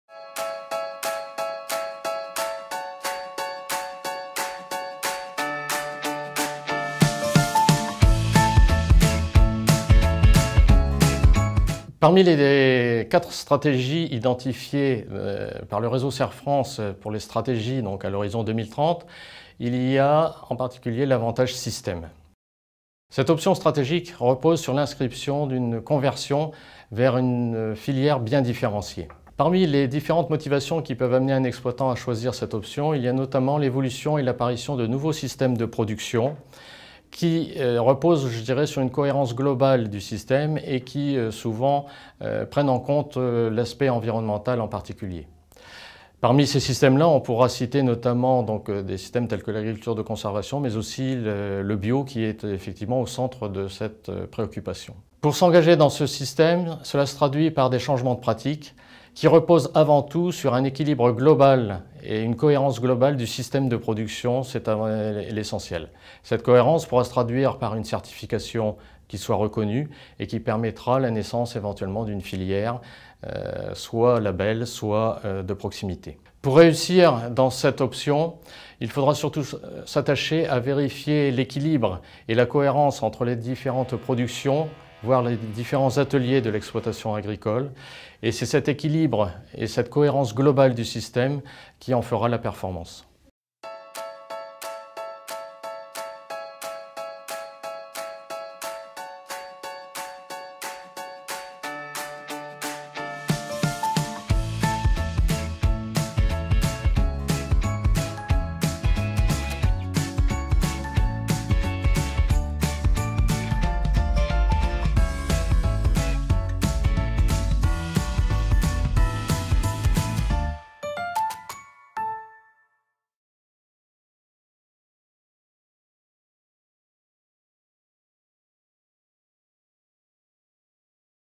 Stratégie 2030 : avantage système
Exploitations agricoles : comment rester dans la course à l'horizon 2030 ?
4 options stratégiques présentées par le groupe de Veille économique agricole Cerfrance.
Dans cette vidéo, l'Avantage système présenté par Thierry Lemaître du Cerfrance Champagne Nord Est Ile-de-France.